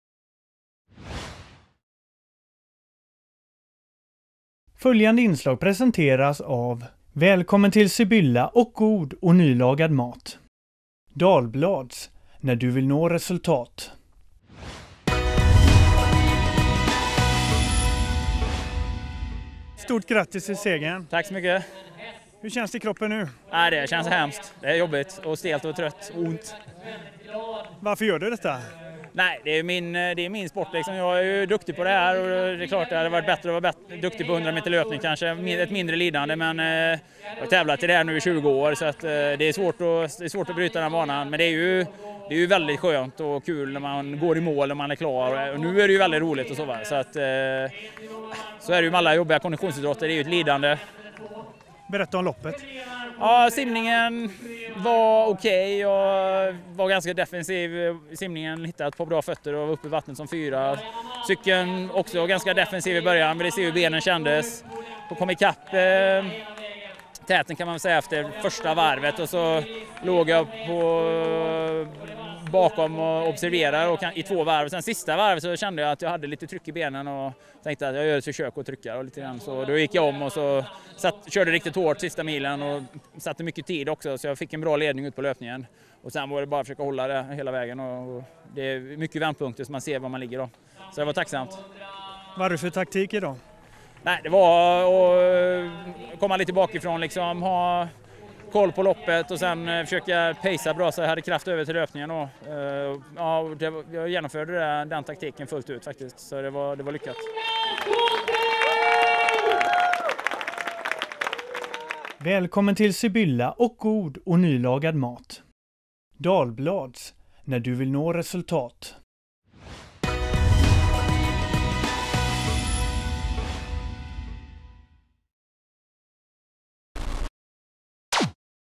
Göteborg Triathlon 2010 - Jonas Colting
Intervju med Jonas Colting, vinnare av Göteborg Triathlon 2010.

Årets upplaga och 25-årsjubiléet av Göteborg Triathlon genomfördes i Hindås strax utanför Göteborg. Triathleterna var på plats och kämpade på OS-distansen 1500m simning, 40 km cykel och 10 km löpning. Det blev en fartfylld tävling som bjöd på många härliga ögonblick.
TV VÄST var på plats för att följa denna kamp i uthållighet, fart och styrka.